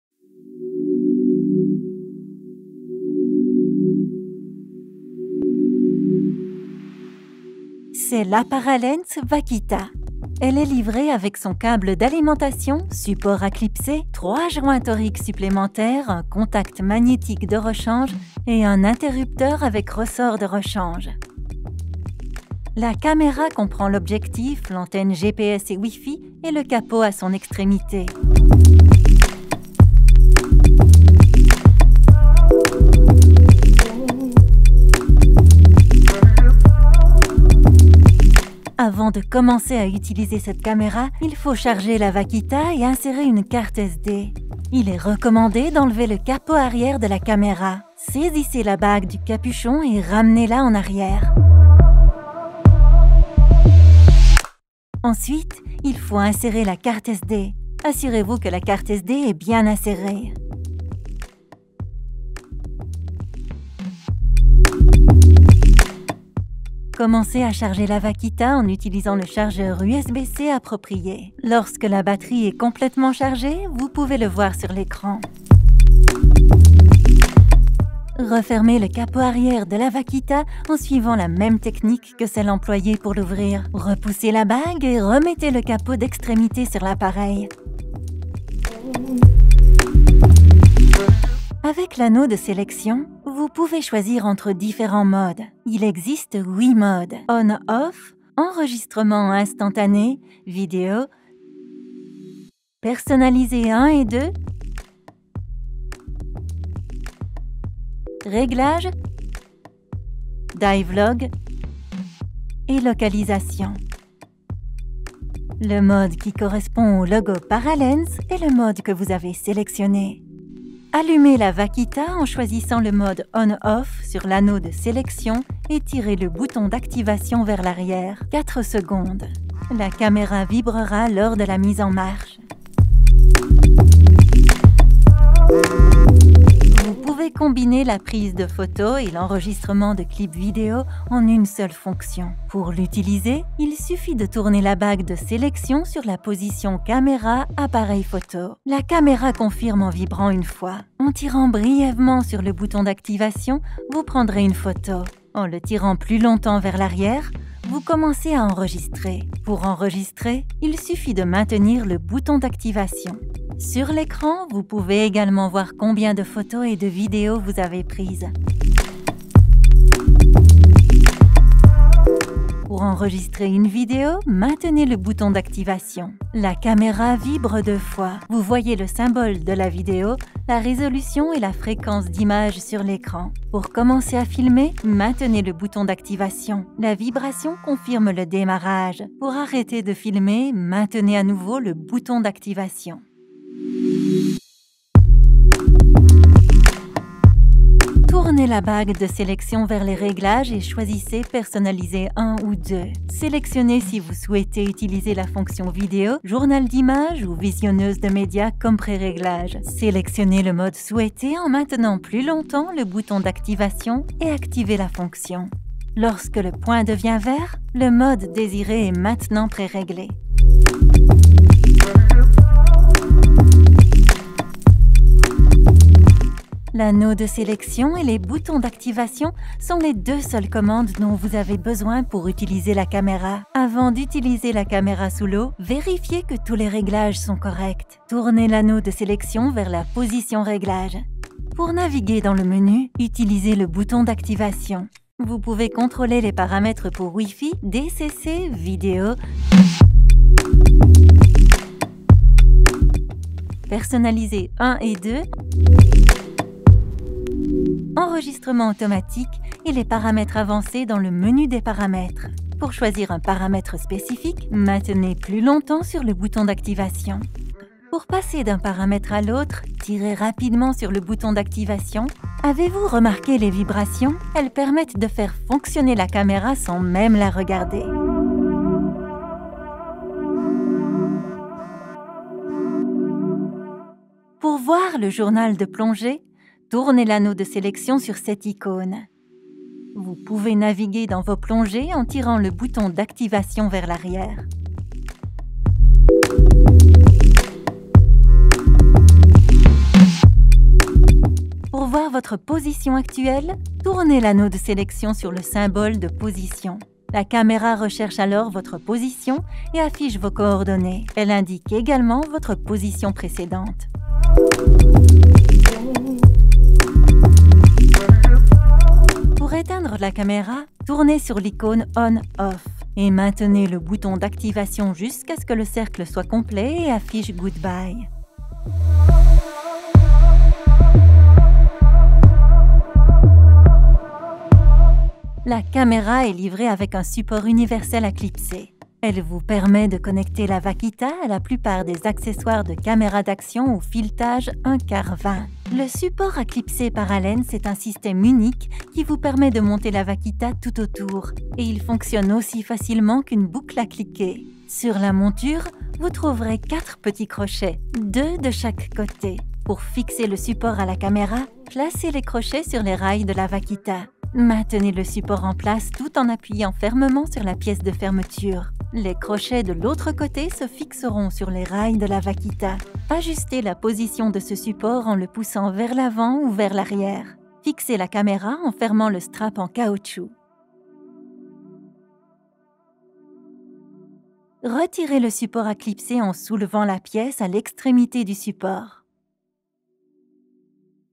Utilisation de la Vaquita
Apprenez à utiliser le Vaquita et son système Click-Mount.

PARALENZ® created an underwater video camera “by divers, for divers” to change the game of underwater videography.